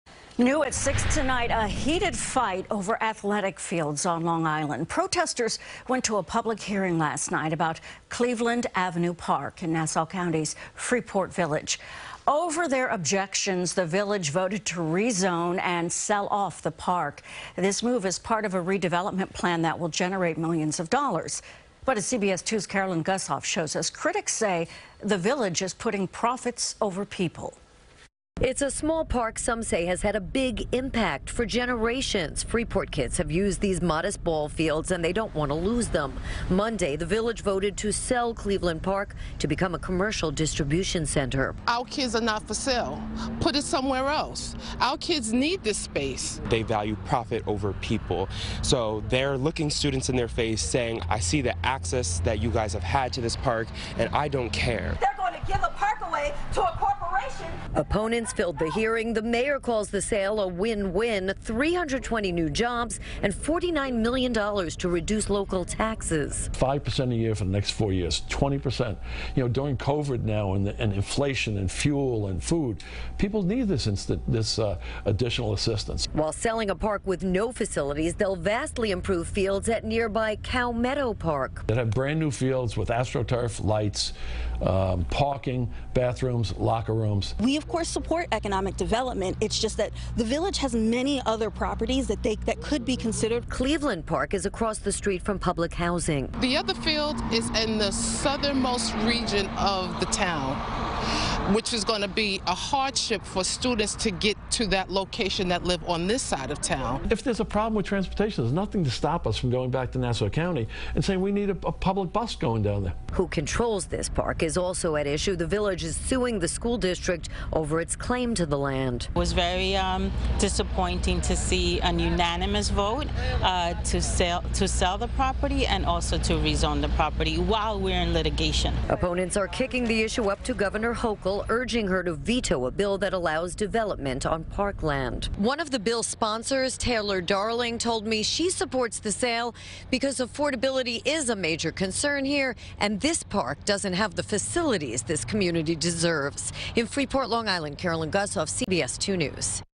Freeport votes to sell park amid criticism from community members
There's a heated fight over athletic fields on Long Island. Protesters attended a public hearing Tuesday night about Cleveland Avenue Park in Nassau County's Freeport Village. CBS2's Carolyn Gusoff reports.